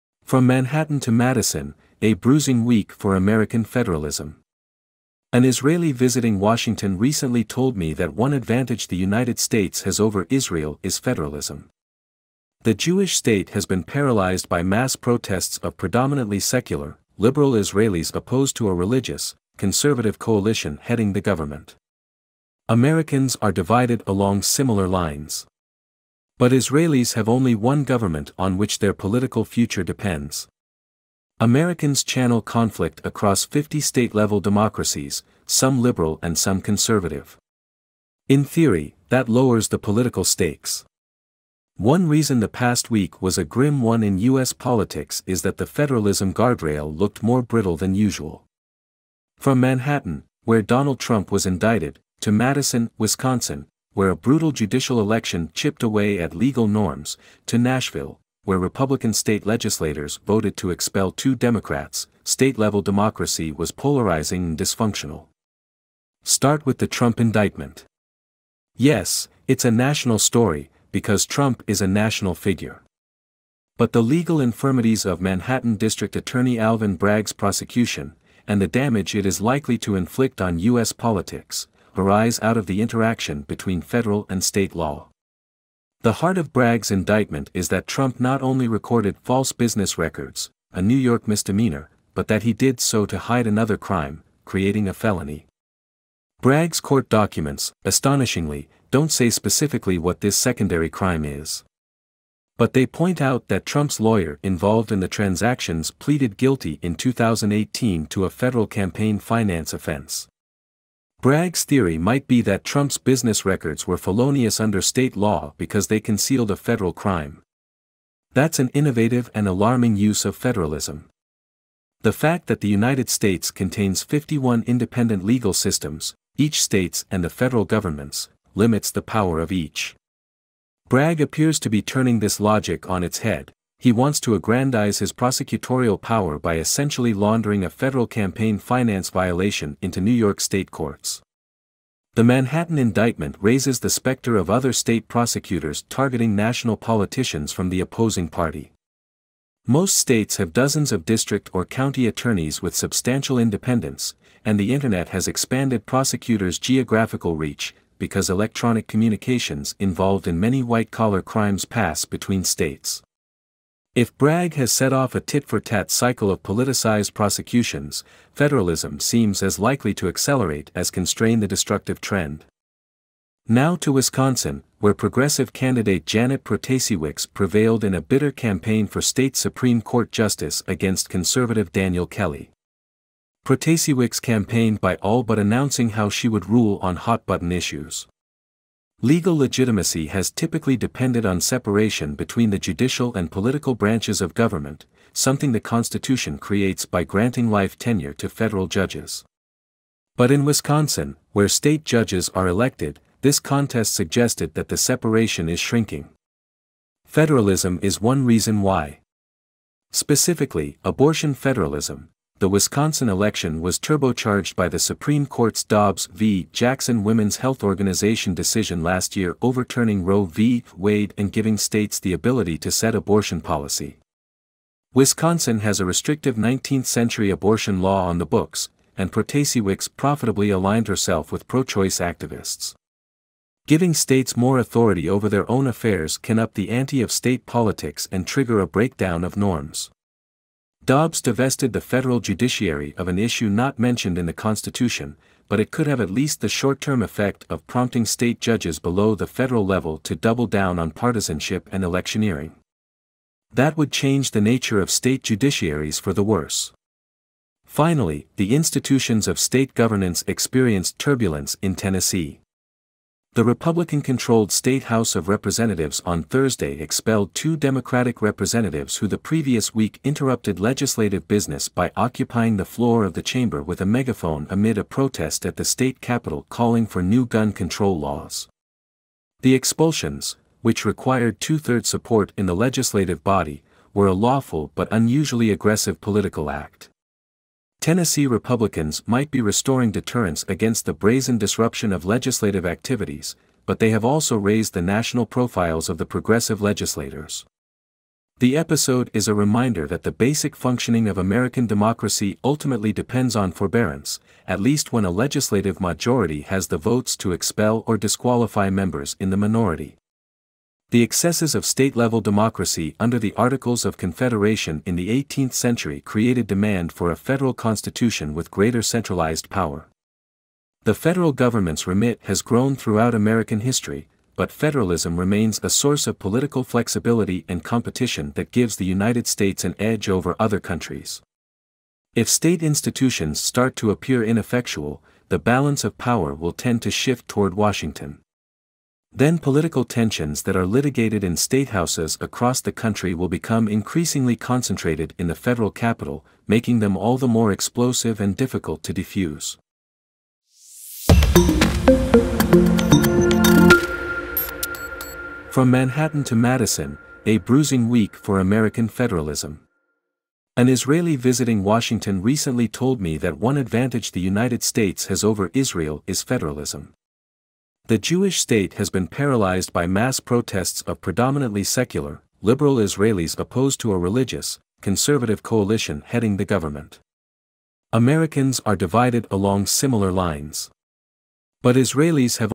FROM MANHATTAN TO MADISON, A BRUISING WEEK FOR AMERICAN FEDERALISM / JASON WILLICK article opinion
FROM MANHATTAN TO MADISON, A BRUISING WEEK FOR AMERICAN FEDERALISM / JASON WILLICK - THE WASHINGTON POST

An Israeli visiting Washington recently told me that one advantage the United States has over Israel is federalism. The Jewish state has been paralyzed by mass protests of predominantly secular, liberal Israelis opposed to a religious, conservative coalition heading the government. Americans are divided along similar lines. But Israelis have only one government on which their political future depends. Americans channel conflict across 50 state-level democracies, some liberal and some conservative. In theory, that lowers the political stakes.